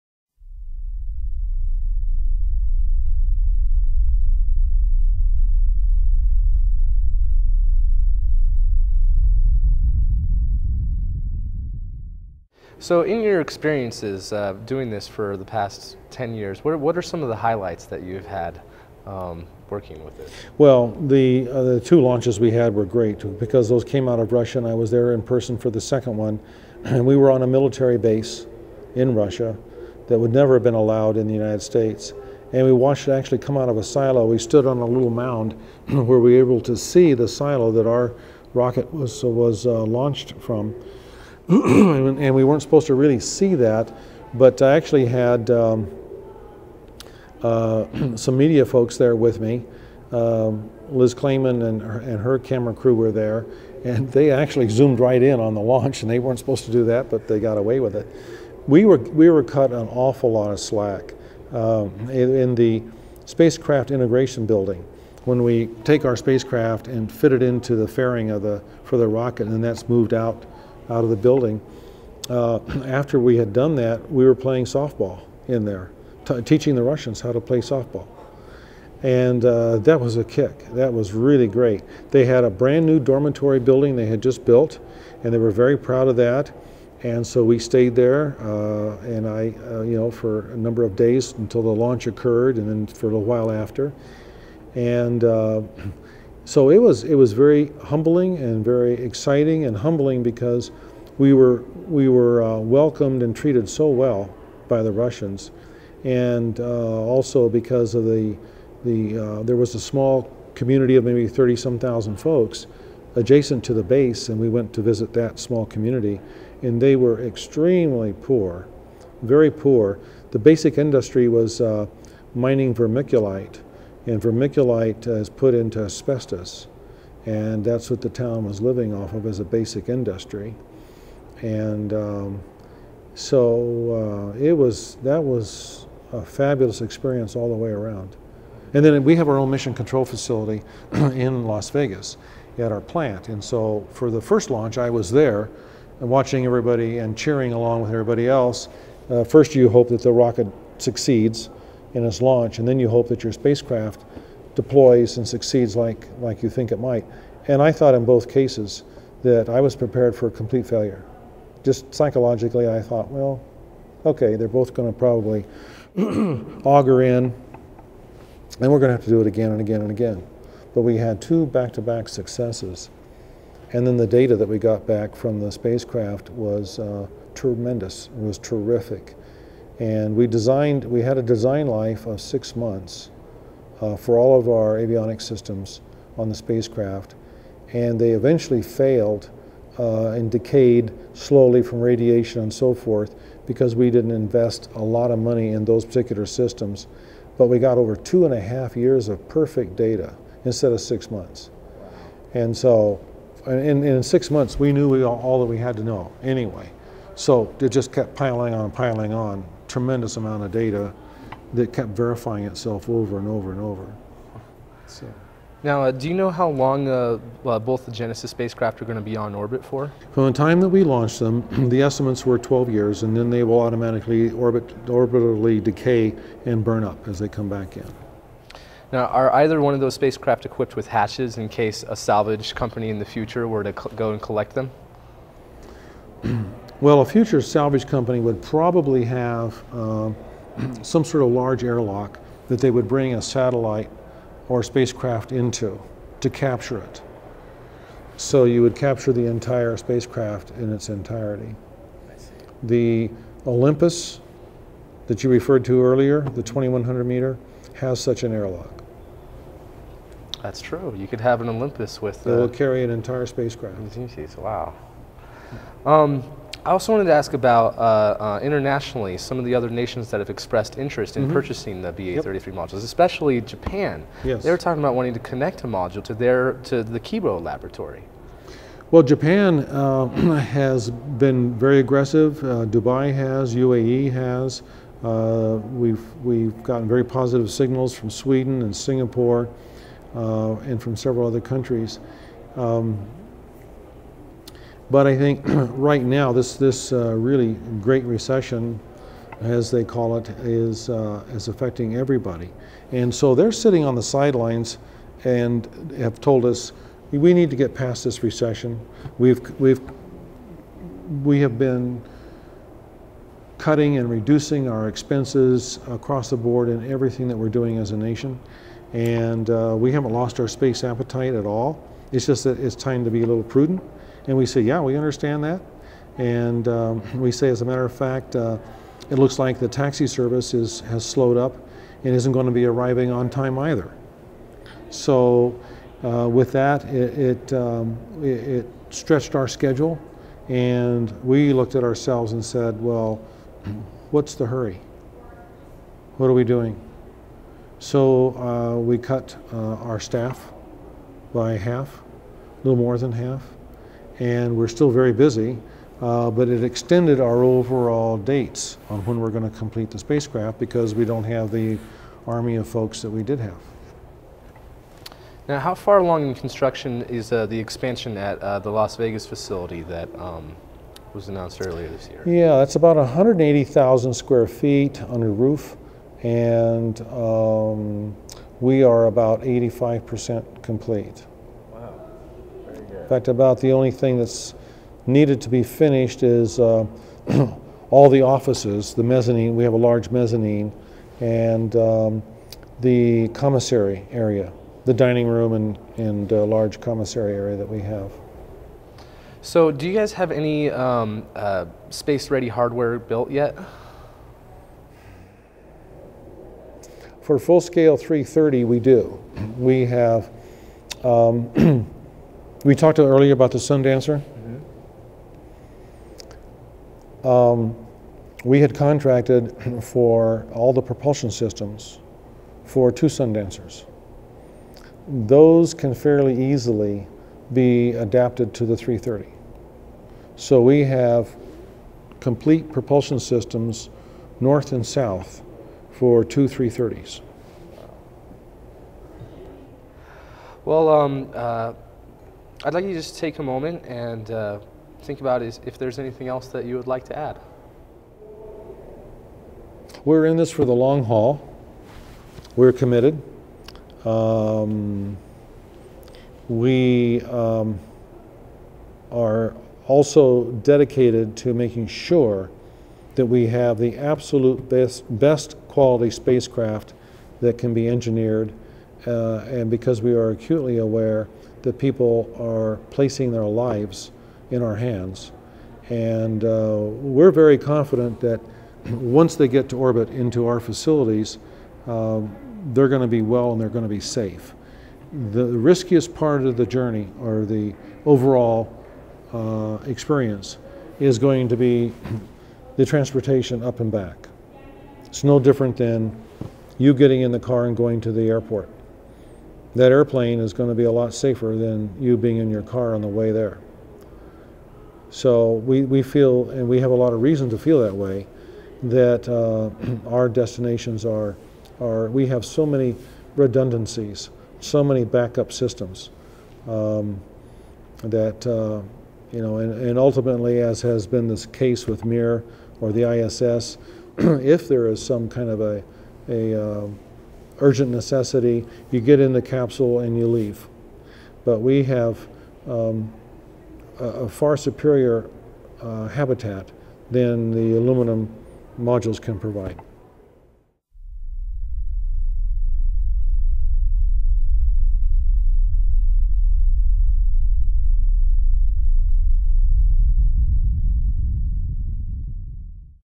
The People of NewSpace: Robert Bigelow, part 4 -- Highlights And Plans
In the last of his 4-part interview with MM, Bob Bigelow shares vignettes of his personal highlights running Bigelow Aerospace, mentions some of the other nations who have expressed interest in purchasing Bigelow modules, discusses the current state of buildout of Bigelow Aerospace headquarters, and more.